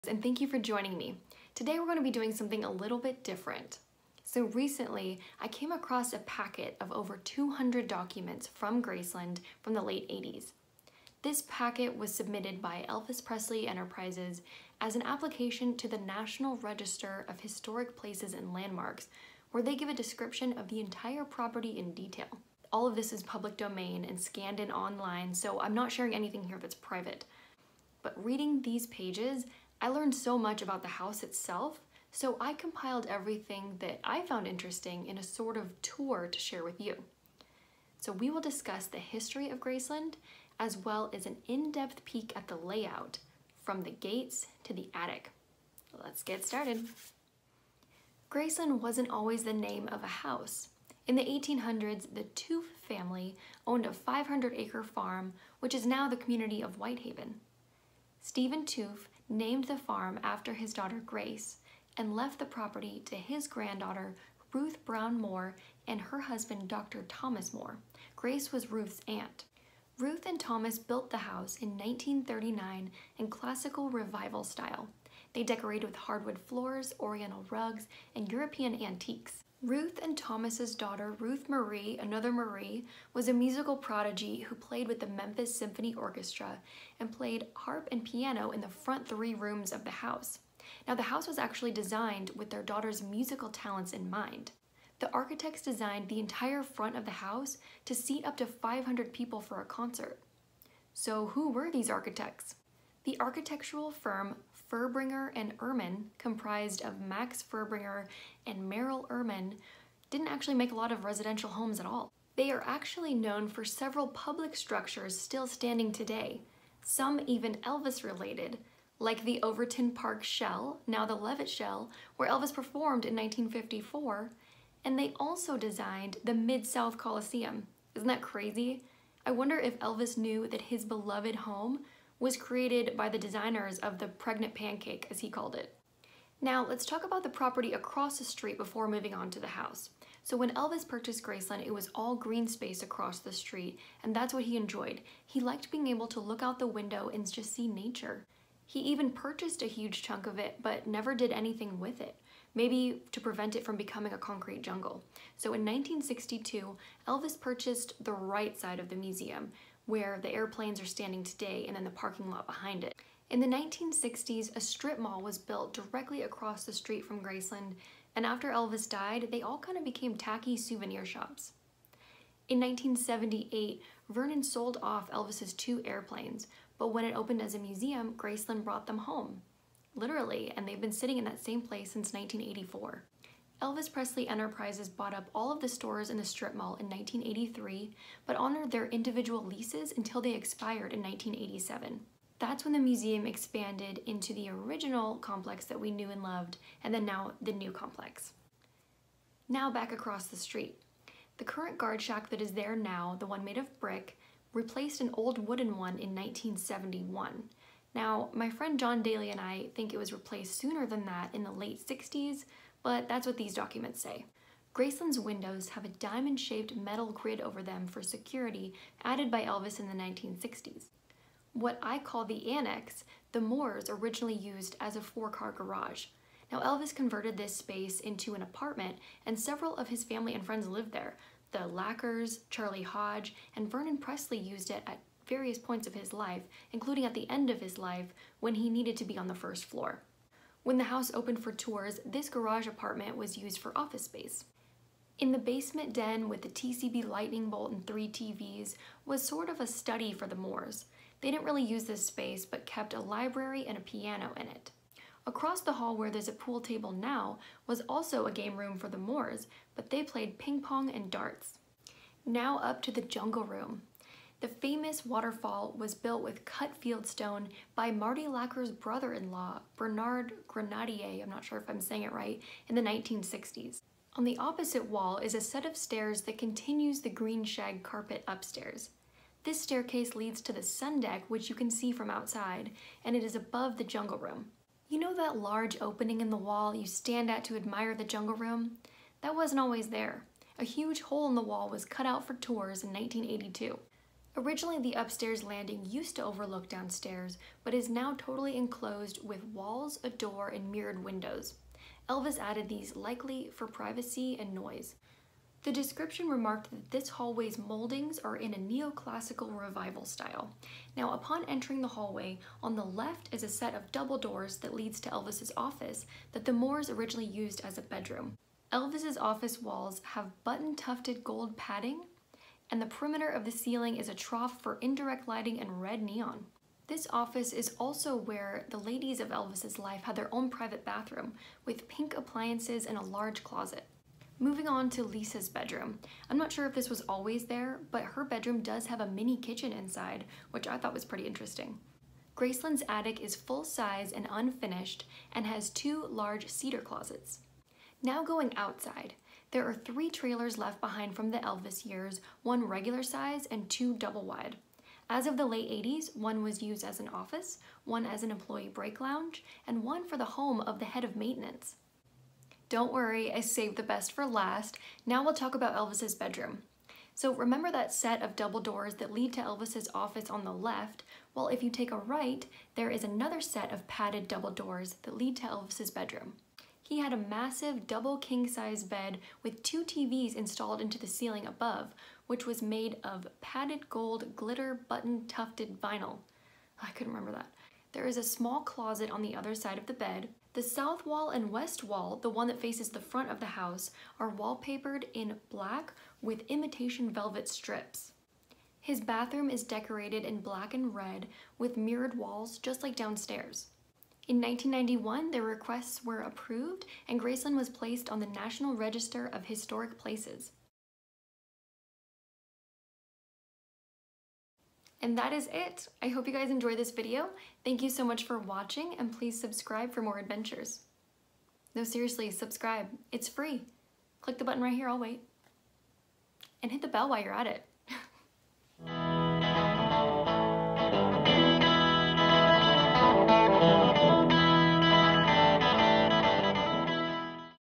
A Secret FILE! | SECRET GRACELAND #2
A Secret FILE! Secret Graceland #2. Let's take a deep dive into all the corners of Elvis Presley's Graceland in Memphis, TN and see what we can find! All Elvis fans welcome!